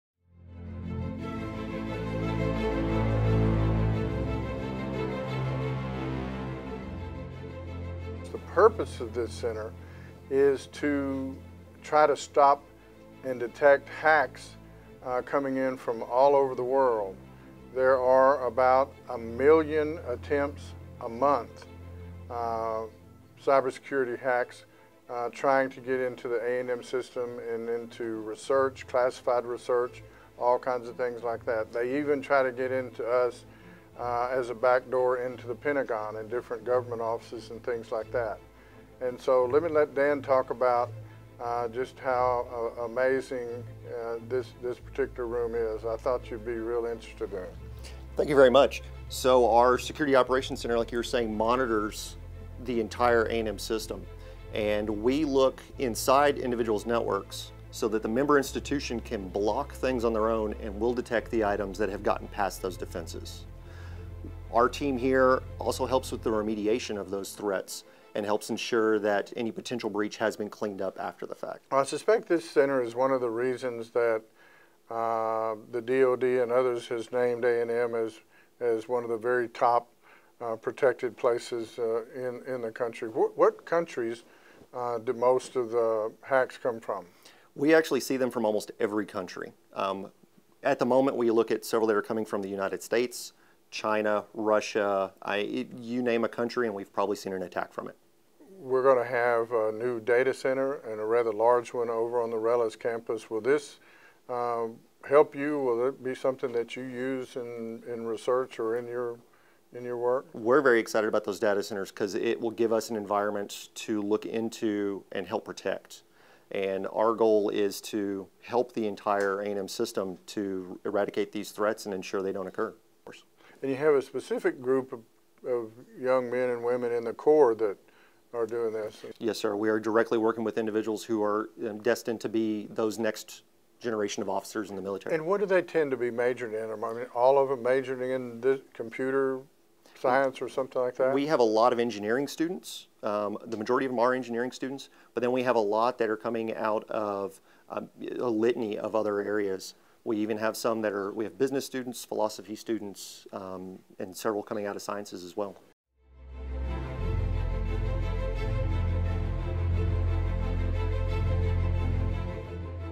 Security Operations Center at The Texas A&M University System
More than one million attempts occur every month by hackers to try to crack into The Texas A&M University System. A real-time of attempted breaches appears in the background of the video. Facilities like the Security Operations Center, or "SOC," fend off these attempts to steal sensitive and classified information.